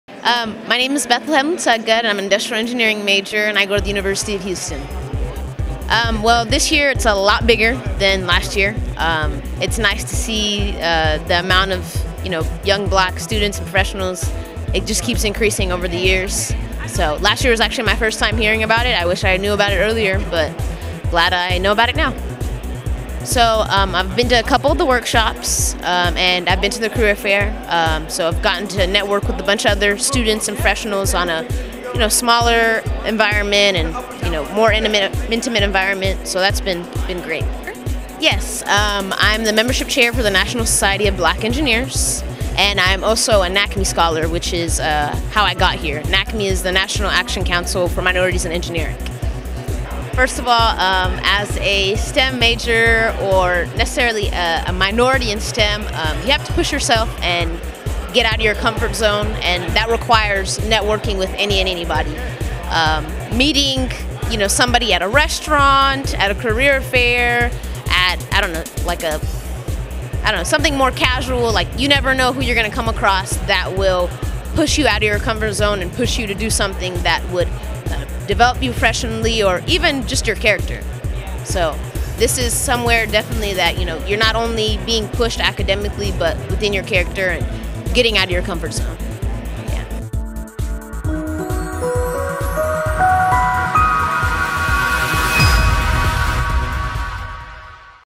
Bethlehem Lulseged Talks BEYA
Industrial Engineering Student, Bethlehem Lulseged from the University of Houston, National Society of Black Engineers (NSBE) Membership Chair, National Action Council for Minorities in Engineering, Inc. Scholar